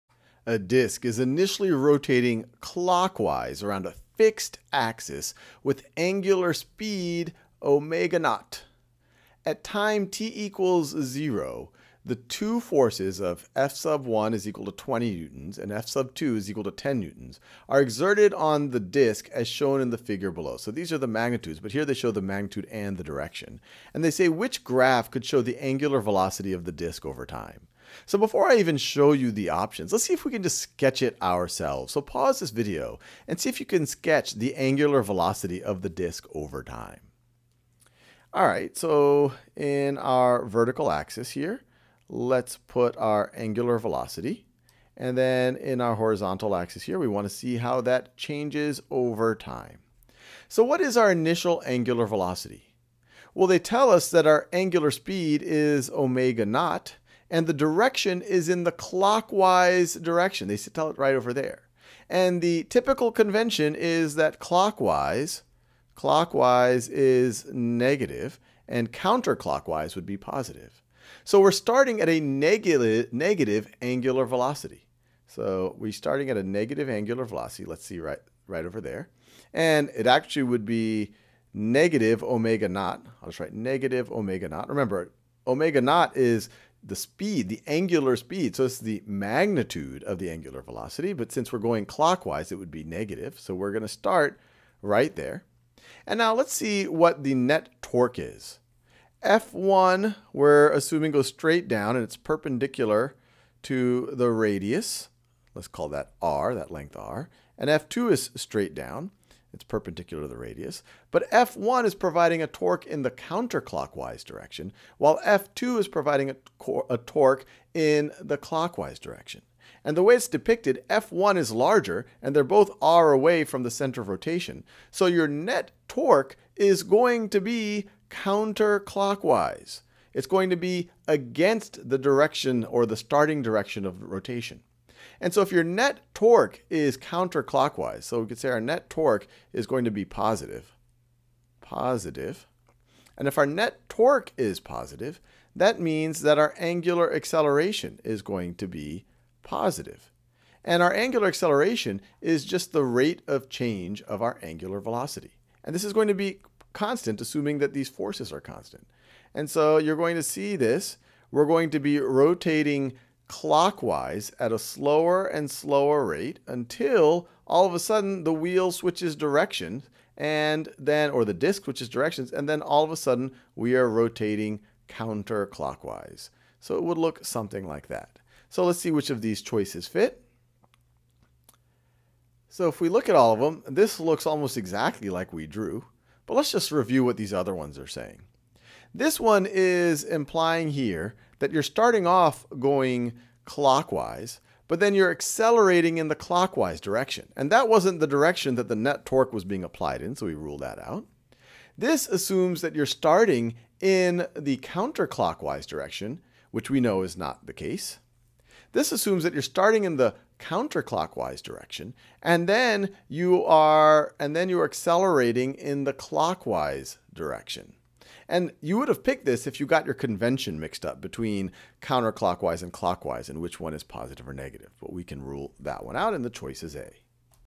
Angular velocity graphs due to multiple torques
Example thinking through how what the angular velocity graph would look like due to torques be applied to a rotating disk.